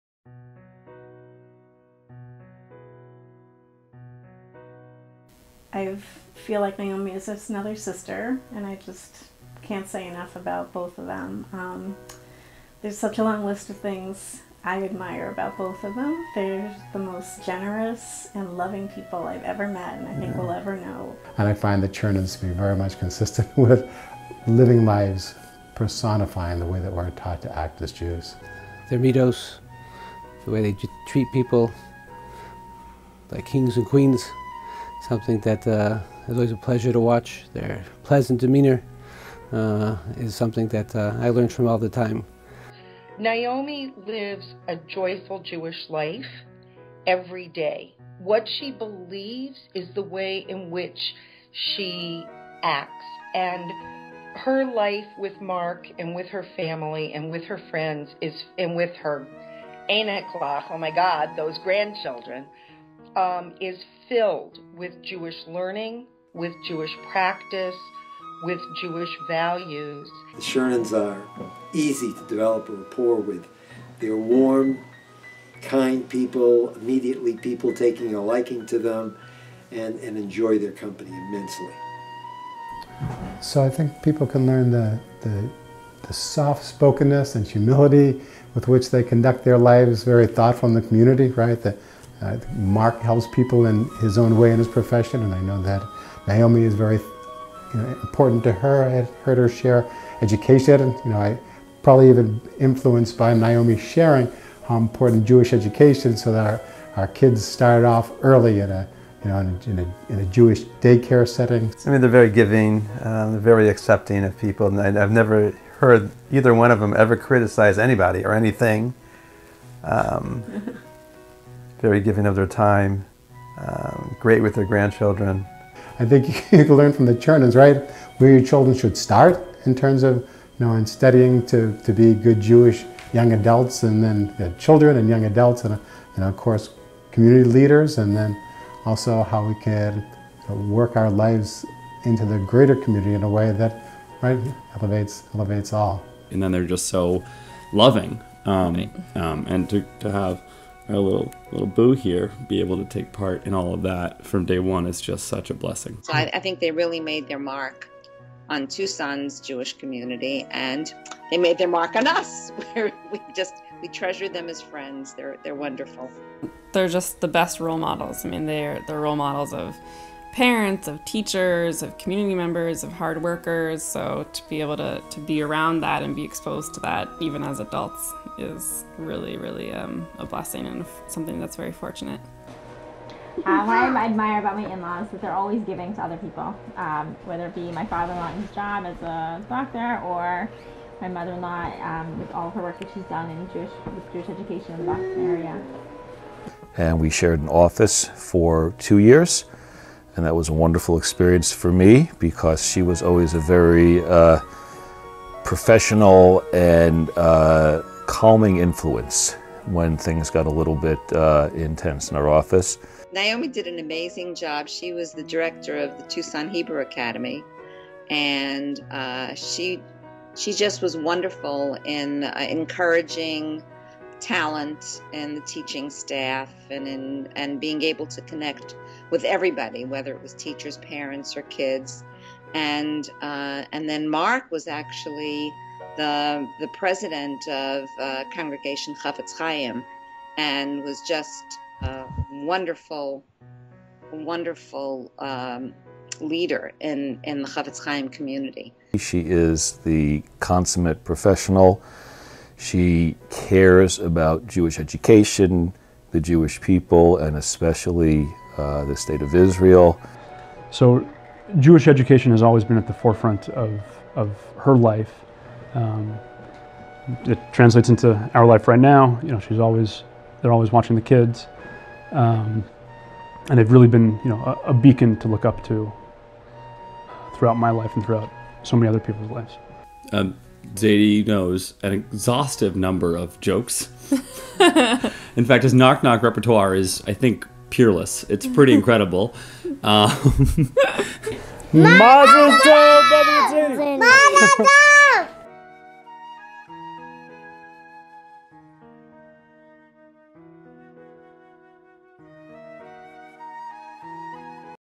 Profiles in Jewish Leadership 2014: Dr. Mark and Naomi Chernin
A video paying tribute to the amazing accomplishments of Mark and Naomi Chernin. This video was shown at the Community Kollel of Sharon 8th Anniversary Banquet Celebration on May 14, 2014.